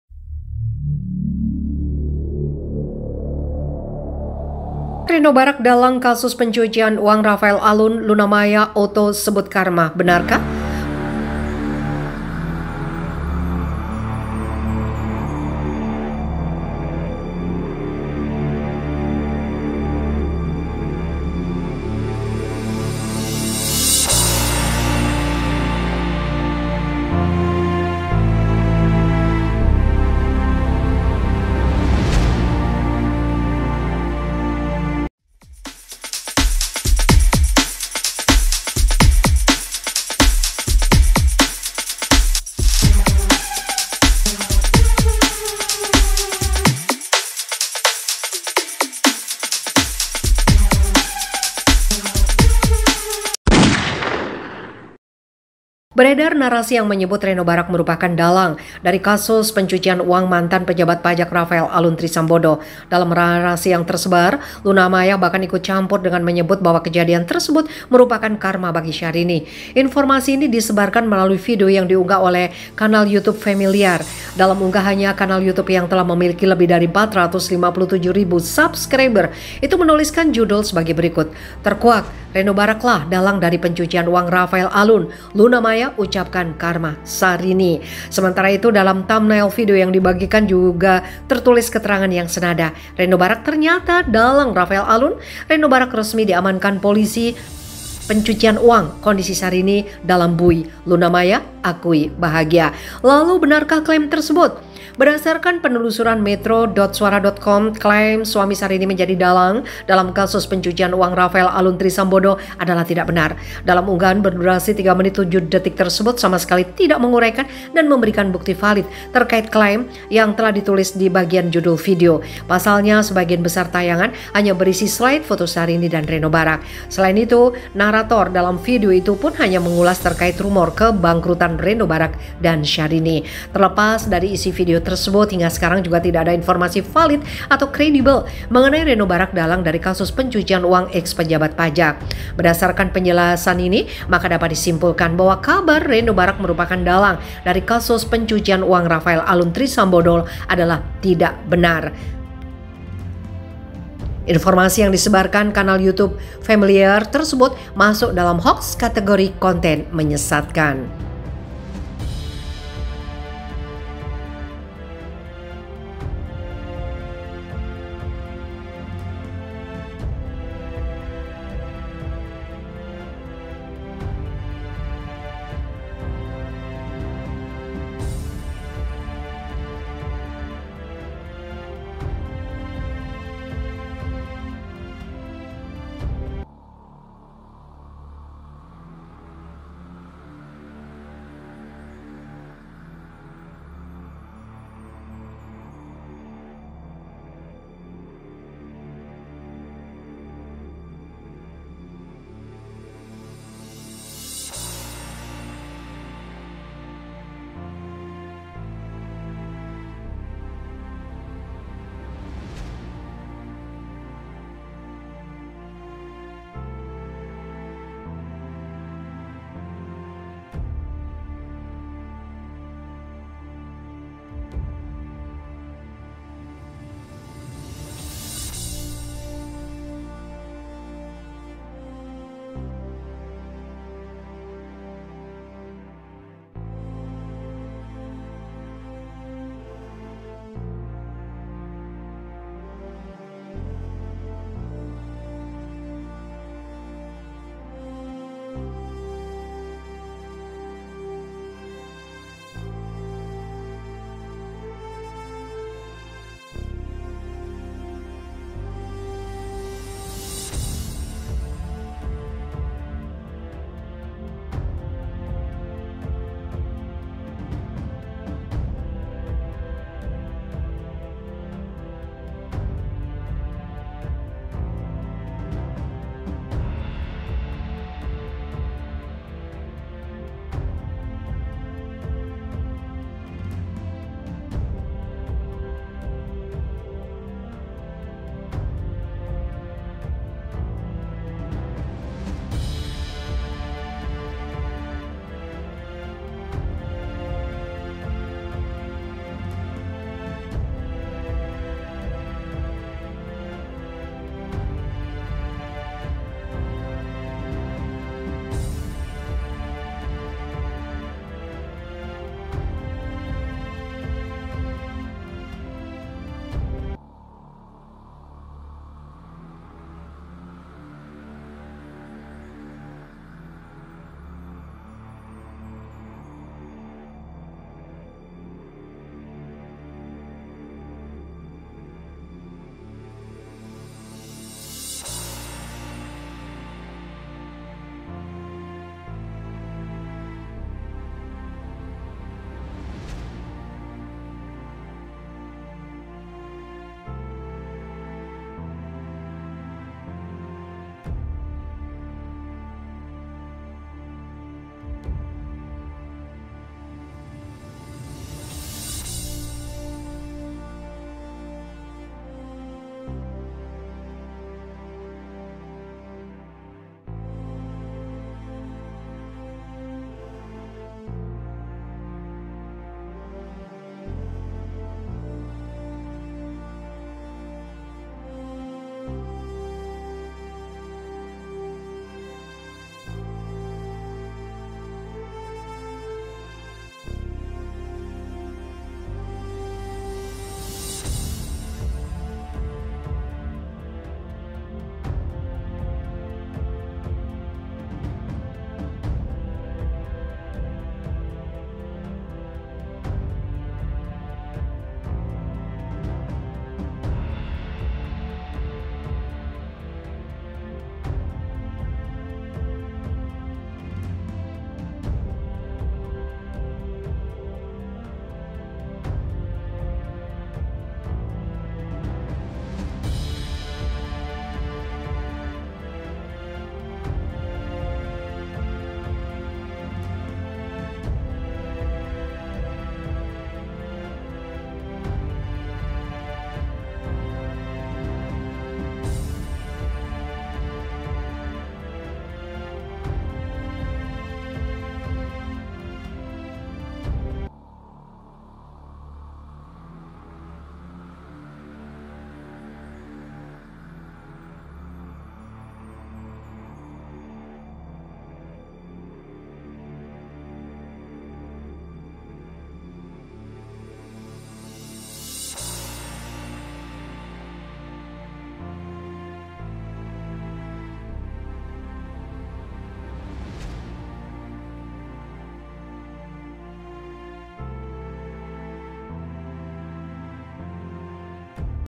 REINO BARACK JD TERSANGKA,SYAHRINI SIAP JANDA,INFOTAINMENT,NEWS
Reino Barack JD Tersangka, Syahrini Siap Janda, Infotainment, News related to Indonesia.

In this video, I'll be discussing the latest news in Indonesia and discussing the role of Reino Barack JD Tersangka and Syahrini Siap Janda in it. I'll also be giving you a brief overview of the recent political developments in Indonesia and what they could mean for the country's future.